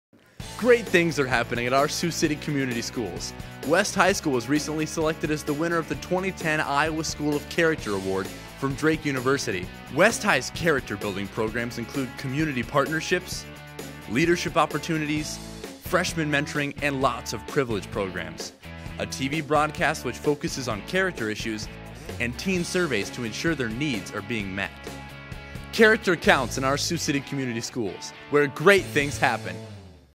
Sioux City Schools_ISSUE 5.wmv
West High School selected as Iowa School of Character